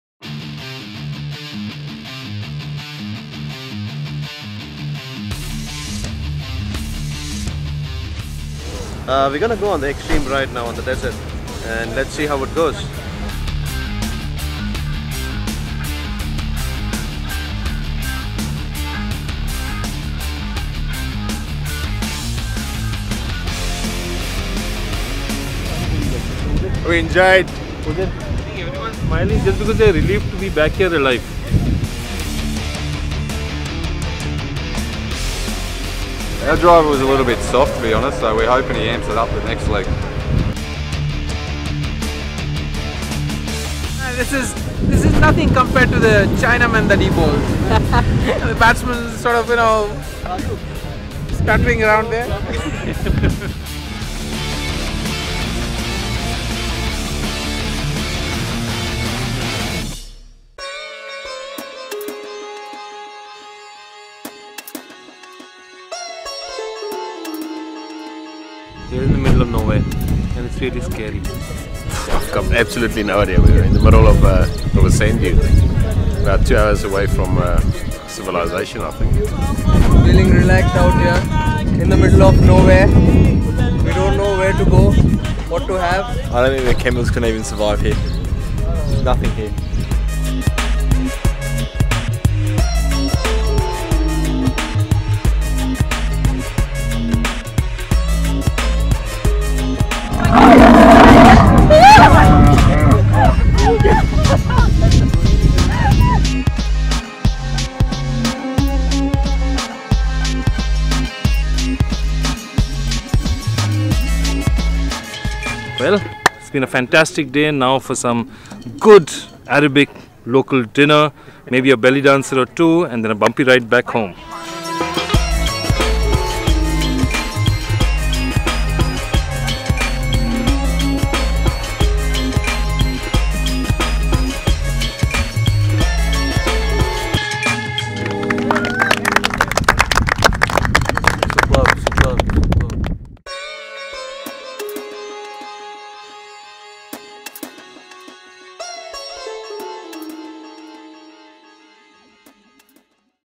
KKR TAKE A WILD RIDE WITH LAMA DESERT SAFARI 2014
KKR the winners of IPL 2014 in Lama Desert safari